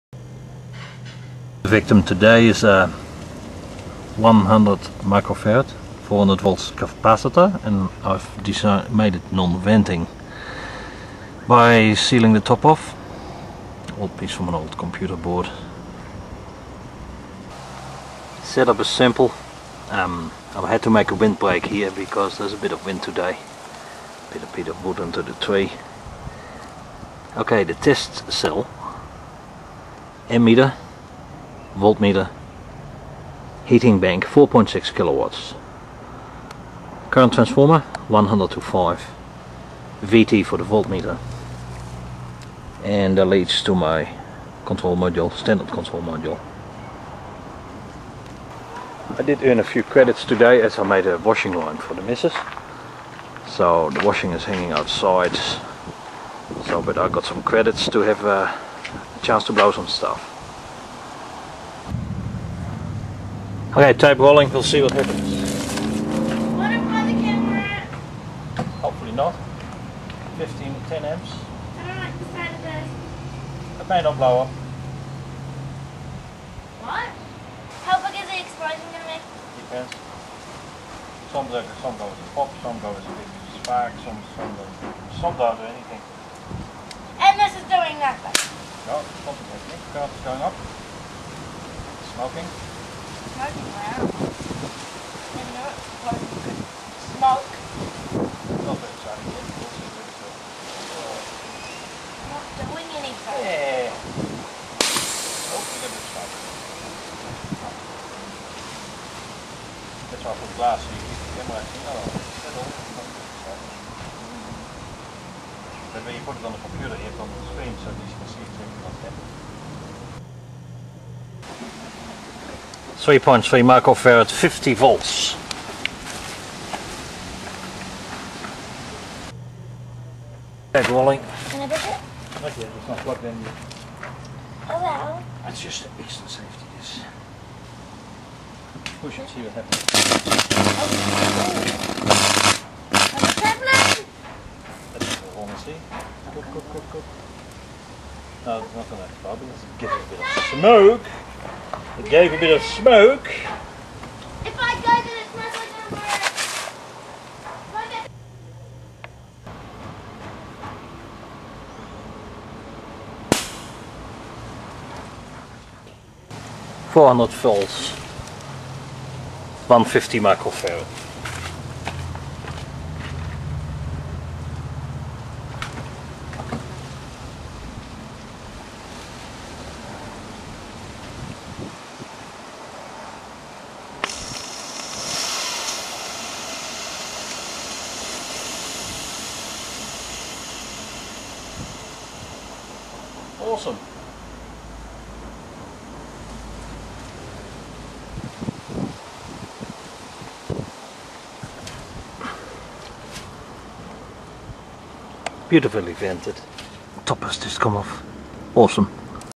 Guy Fawkes Capacitors venting 1
A few of capacitors were put to destructive testing with excessive voltage outside their normal operating parameters.